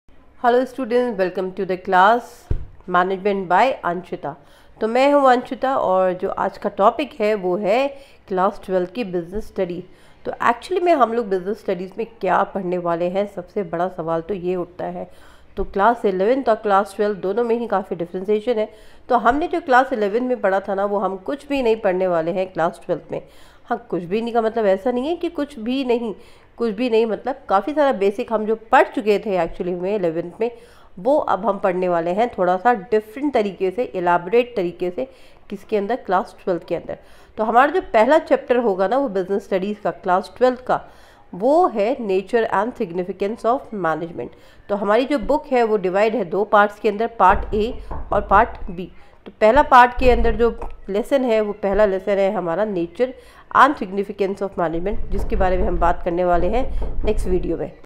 Welocme to our Class business class12 introduction welcome teaching passion lovetoteach #24
This is my first video of introductory session. I am management lecturer .Love to teach my students. Since 2008 I am teaching always keen to learn new for my student’s.